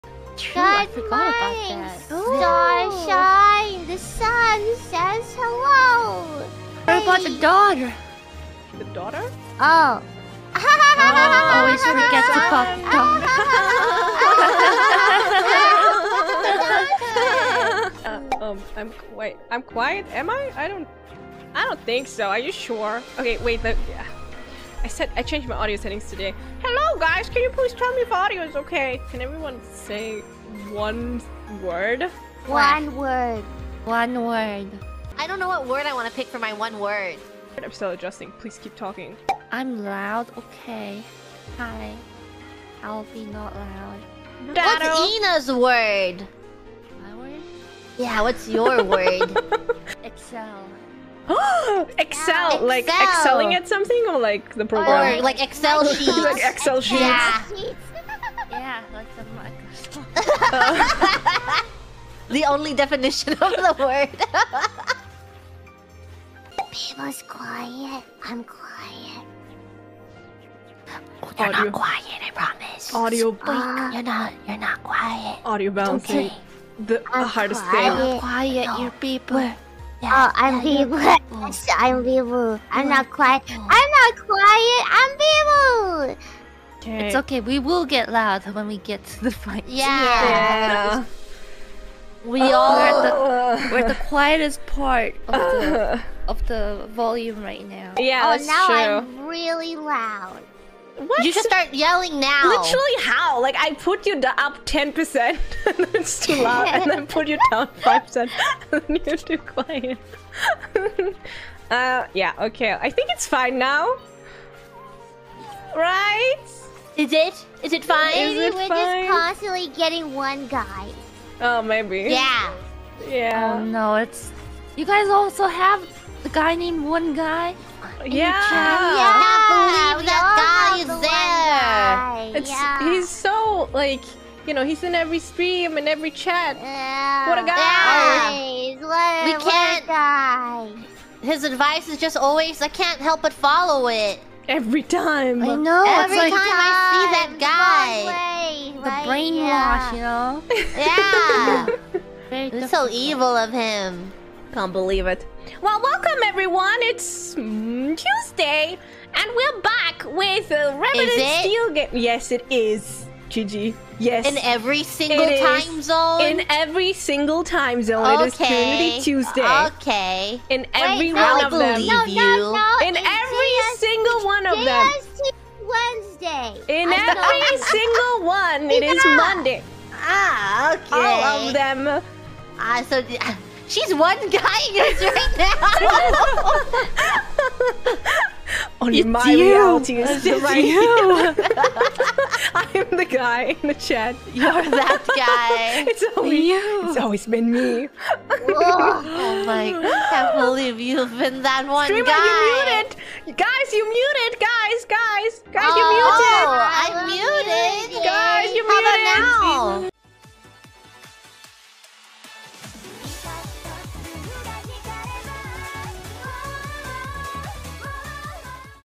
𝗚𝗶𝗴𝗶, 𝗜𝗻𝗮, 𝗮𝗻𝗱 𝗕𝗶𝗯𝗼𝗼 𝗿𝗲𝗮𝗹𝗶𝘇𝗲𝗱 𝘁𝗵𝗮𝘁 𝗖𝗲𝗰𝗶𝗹𝗶𝗮 𝗶𝘀 𝘁𝗵𝗮𝘁 𝗢𝗻𝗲 𝗴𝘂𝘆 𝗶𝗻 𝗲𝘃𝗲𝗿𝘆𝗼𝗻𝗲'𝘀 𝗰𝗵𝗮𝘁 |Hololive
Hololive
Hololive ch
hololive reaction
hololive animation
hololive english
Holocure
holojustice

ED ART by @Tkminnm from Twitter -

BGM used
DOVA-SYNDROME